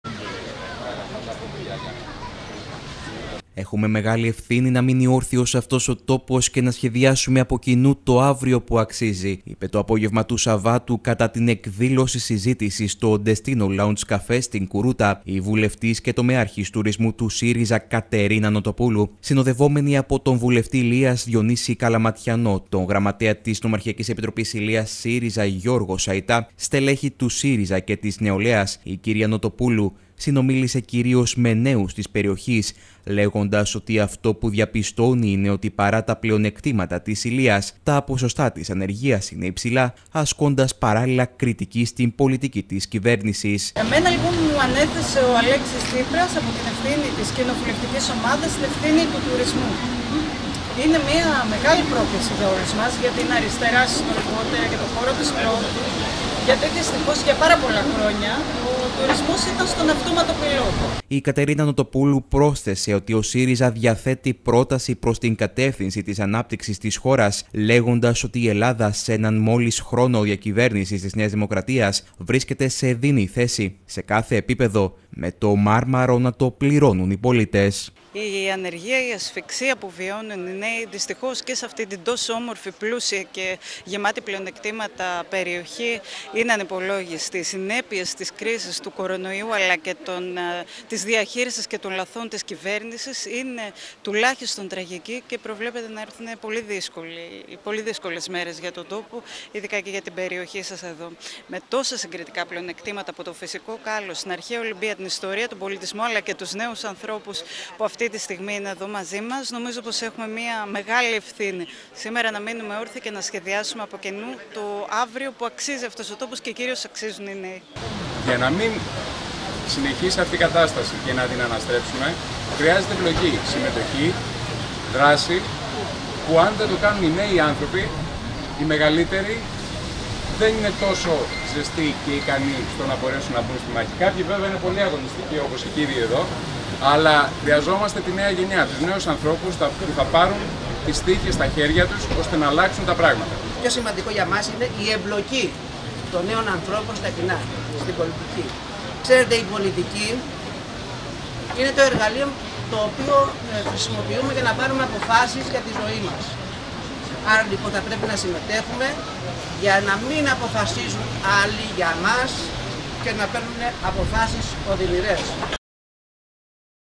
Στην Κουρούτα η Κατερίνα Νοτοπούλου
Στην Ηλεια βρεθηκε το περασμενο Σαββατο η βουλευτης και τομεαρχης τουρισμου του ΣΥΡΙΖΑ, Κατερινα Νοτοπουλου. Η βουλευτής βρέθηκε και στην Κουρούτα οπου συνομίλησε με νέους της περιοχής...Όπως τόνισε, αυτο που διαπιστωνει ειναι οτι παρα τα πλεονεκτηματα της ηλειας, τα ποσοστα της ανεργιας ειναι υψηλα, ασκωντας παραλληλα κριτικη στην πολιτικη της κυβερνησης.